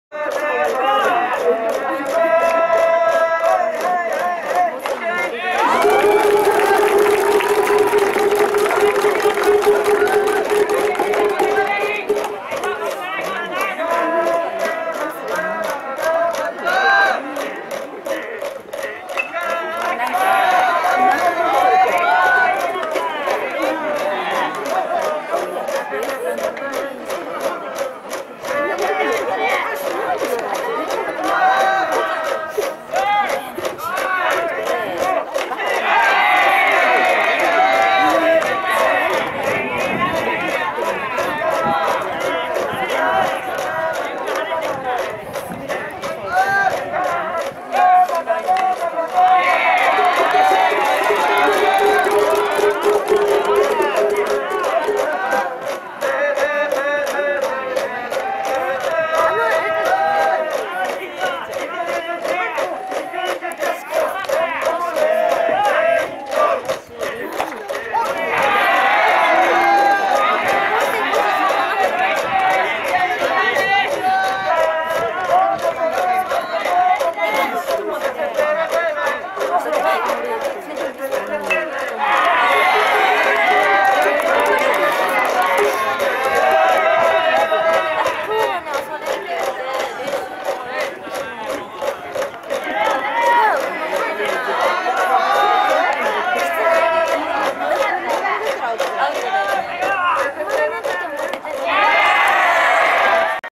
170429 仙台城南・塩釜寛大のピッチング
2017年4月29日　春季宮城県大会南部地区2回戦　対白石高校戦